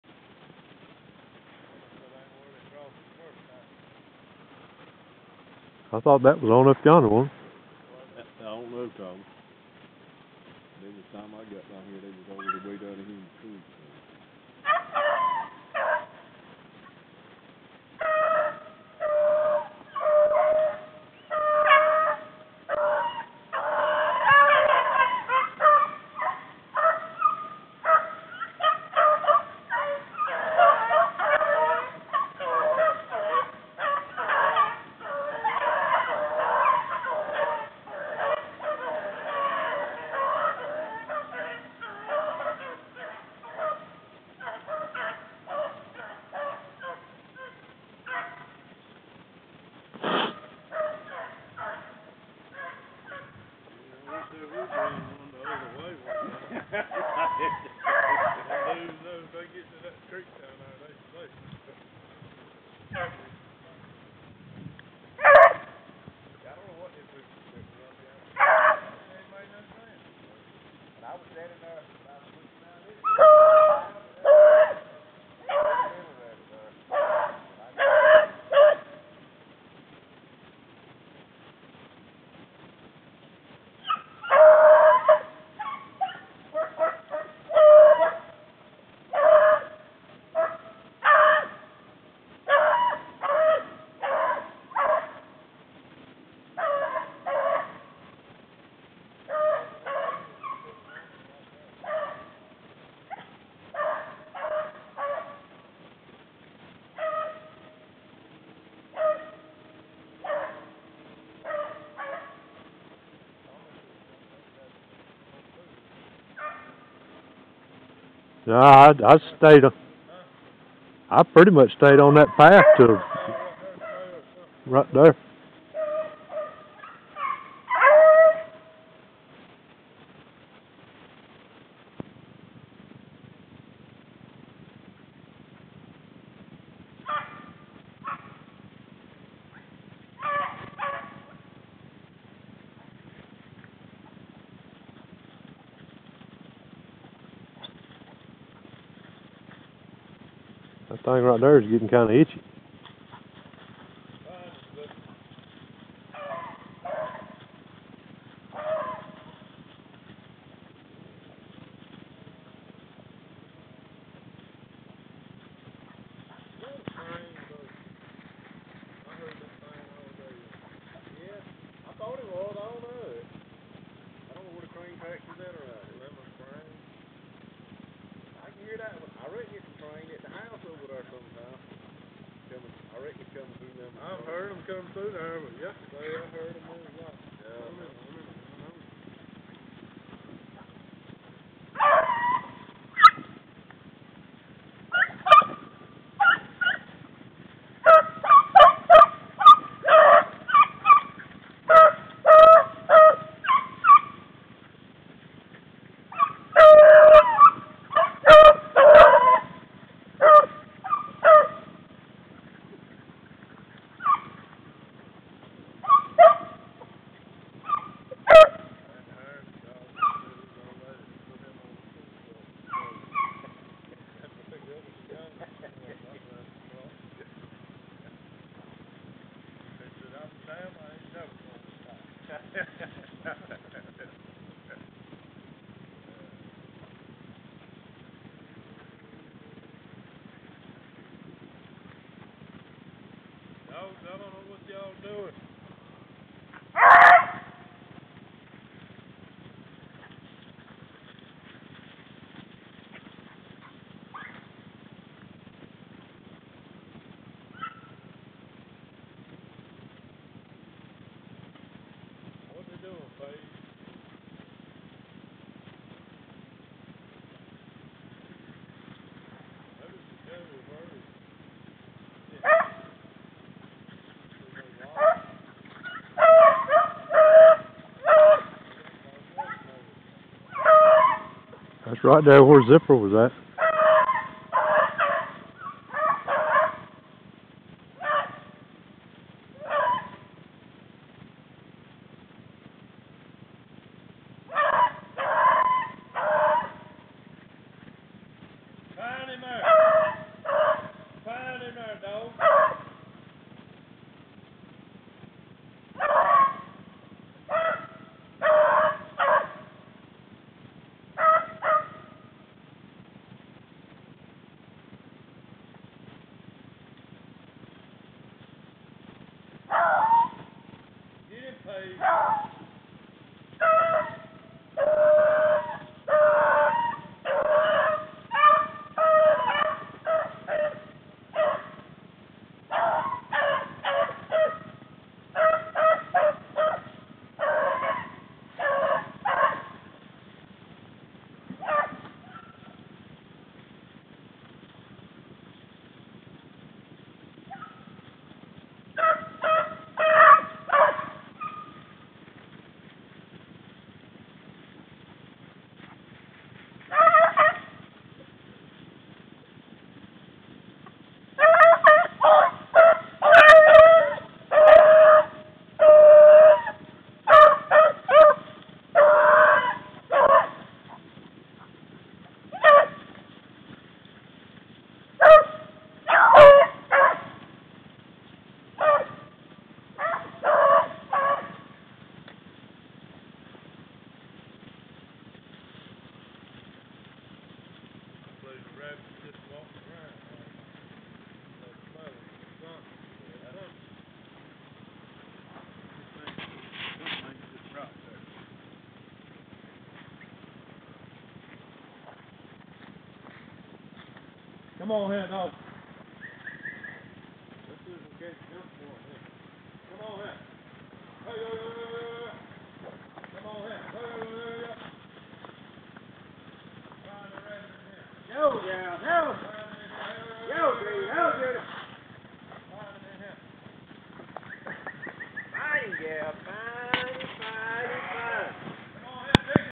rabbit hunting with jimmy cameron
hunting rabbits in harnett county, north carolina